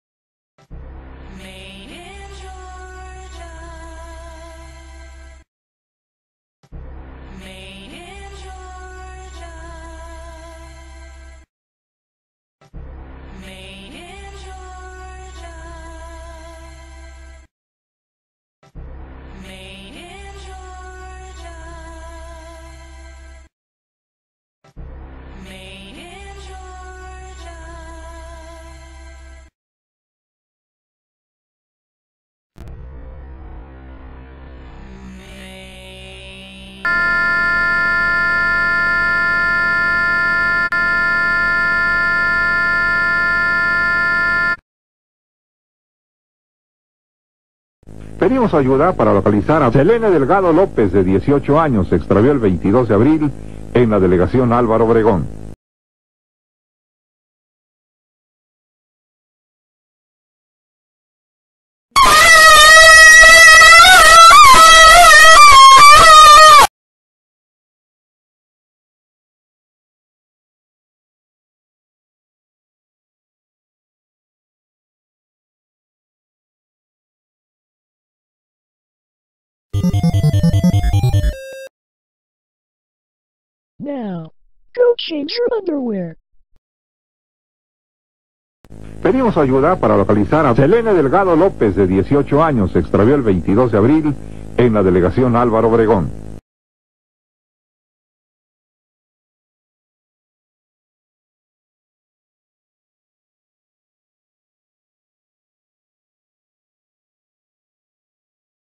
Made in Georgia NIGHTMARE 5 Featuring Selene Delgado Lopez (13124B)
The infamous Georgia Peach logo is back for another NIGHTMARE series video. And this time, she brings the AI generated head of Selene Delgado Lopez to join the fun.

WARNING: Do not watch if you're in a dark room and/or have bathroom issues. Video may cause sudden realease of fecal matter.

End wokeism. Stop weaponized geoengineering.

13124B